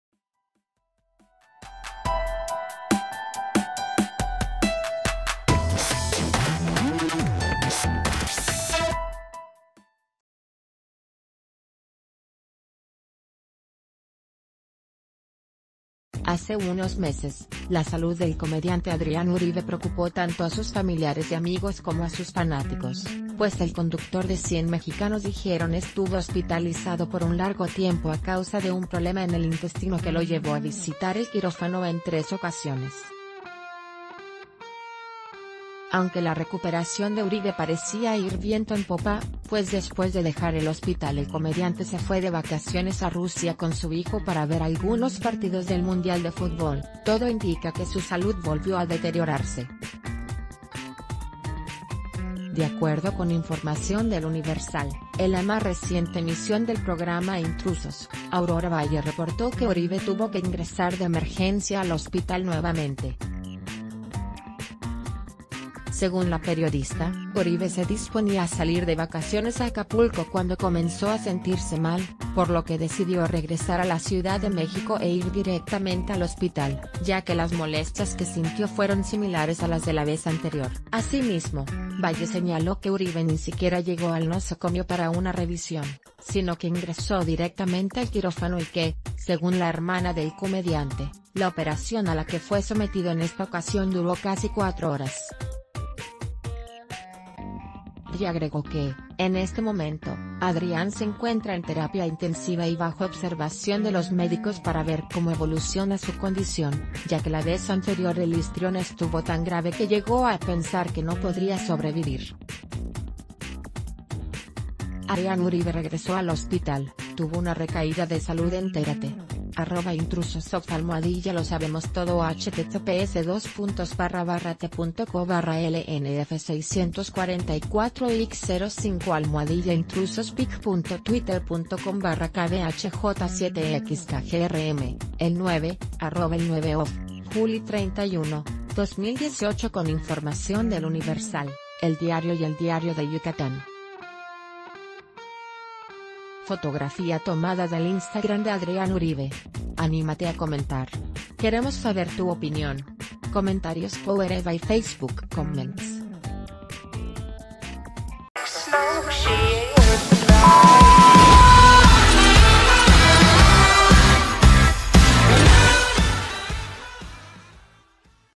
Alarma en Televisa: Adrián Uribe regresa a terapia intensiva (FOTOS)
Hace unos meses, la salud del comediante Adrián Uribe preocupó tanto a sus familiares y amigos como a sus fanáticos, pues el conductor de 100 Mexicanos Dijieron estuvo hospitalizado por un largo tiempo a causa de un problema en el intestino que lo llevó a visitar el quirófano en tres ocasiones. Aunq...